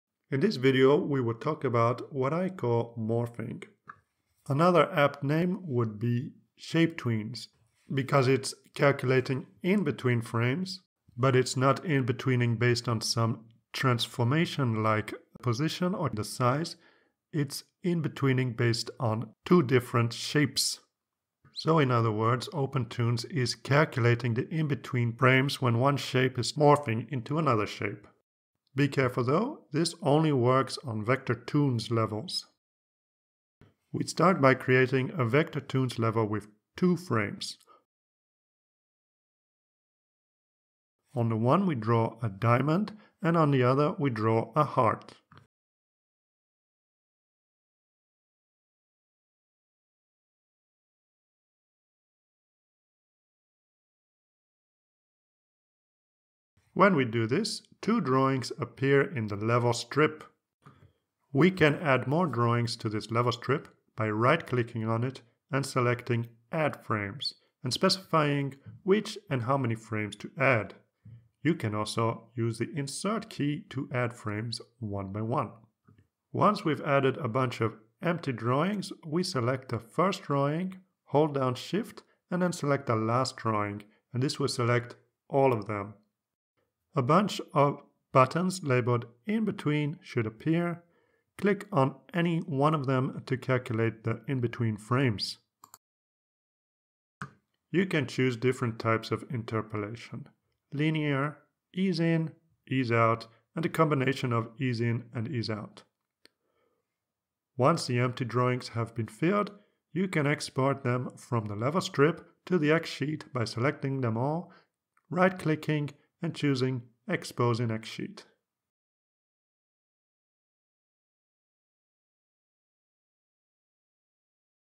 Opentoonz Inbetweening Shapes (Morphing)
In this video we level to use the level strip to create a animated transition from one shape to another. In other words, the animation with morph one shape into another.

This video is part of a course. You can find the full course on Udemy and Skillshare.